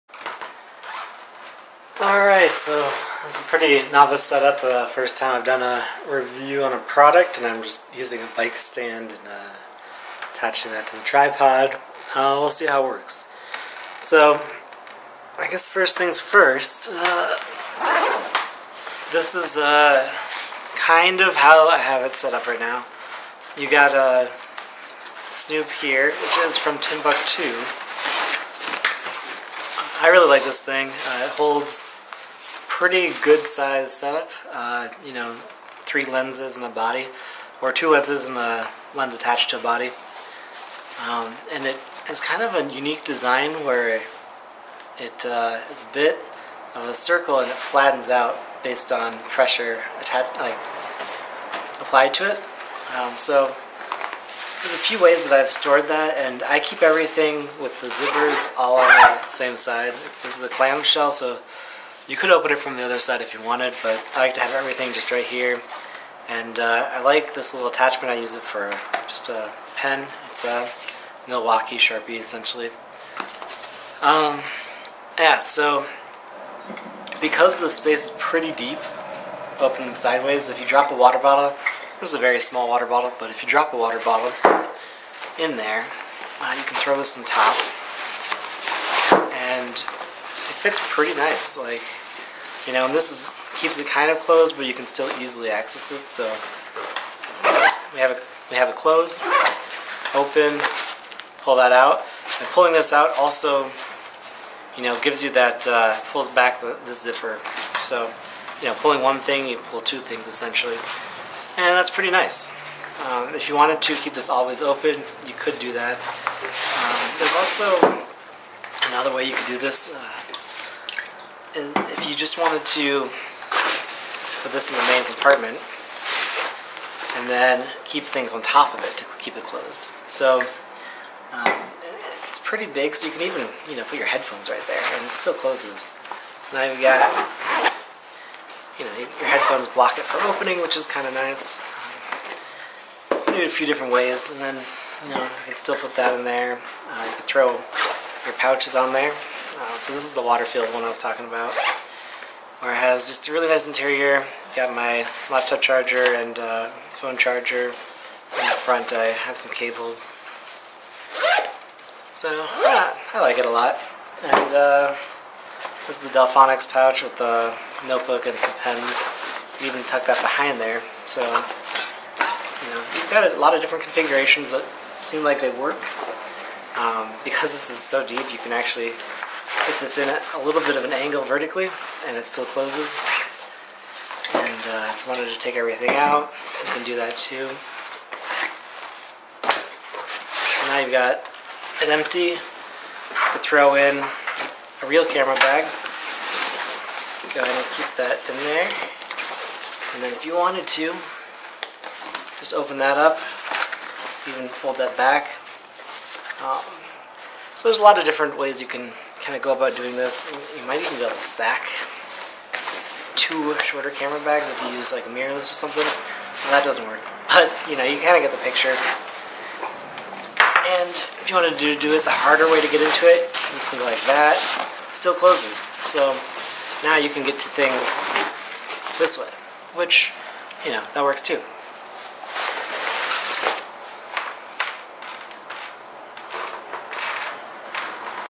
Evergoods Civic Panel Loaded 24 (CPL24) Lightning Review
There was talk about using this as a camera bag. This review covers the different configurations I have used to load out the CPL24 as a camera backpack.